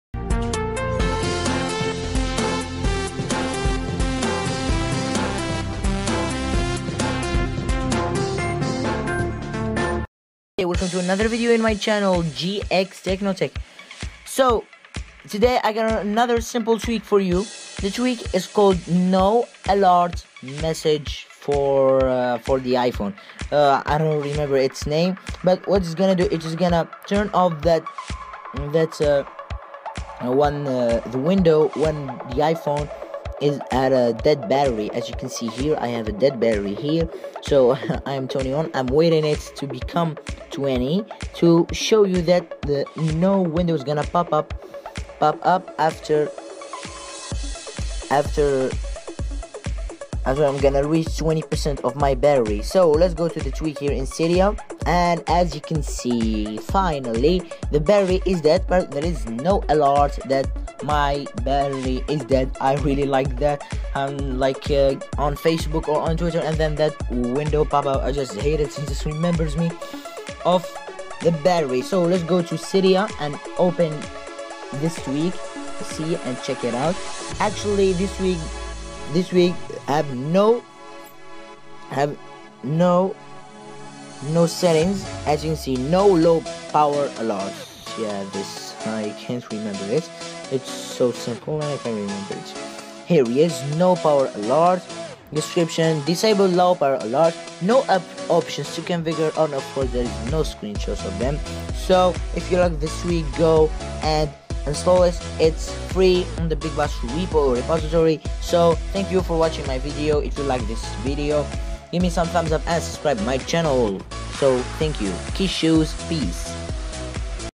iOS 7 tweaks NO LOW POWER ALERT
To turn off that poping window when your battery is dead like 20% or 10%!!!
BigBoss: Free